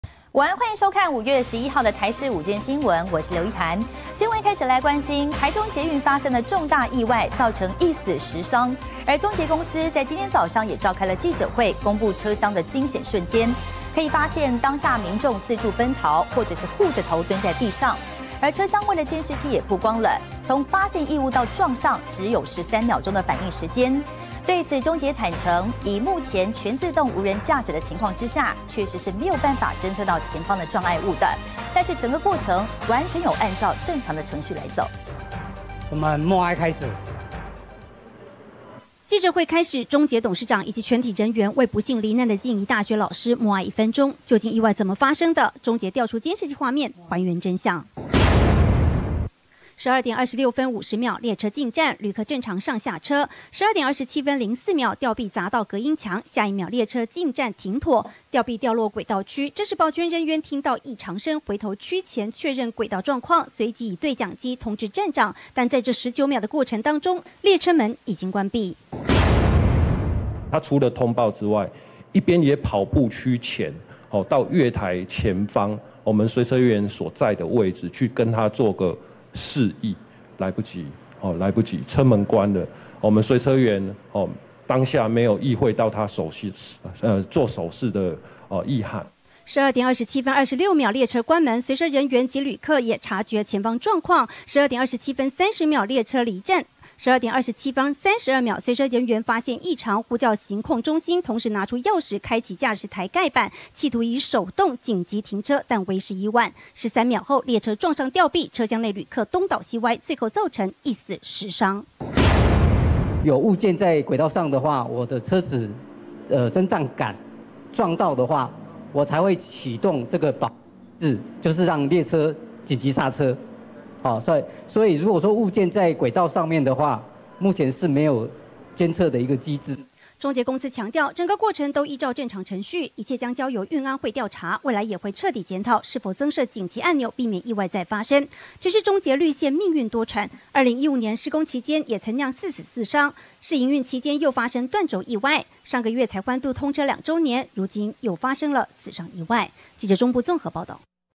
向罹難者默哀!中捷今公布影像 還原事發瞬間
台中捷運發生重大意外，造成一死10傷，中捷公司上午召開記者會，公布車廂驚恐瞬間，當下民眾奔逃或護頭蹲在地上。車廂外監視器也曝光，從發現異物到撞上只有13秒。中捷坦承，以目前全自動無人駕駛的情況下，確實無法偵側前方障礙物，但整個過程，完全有按照正常程序。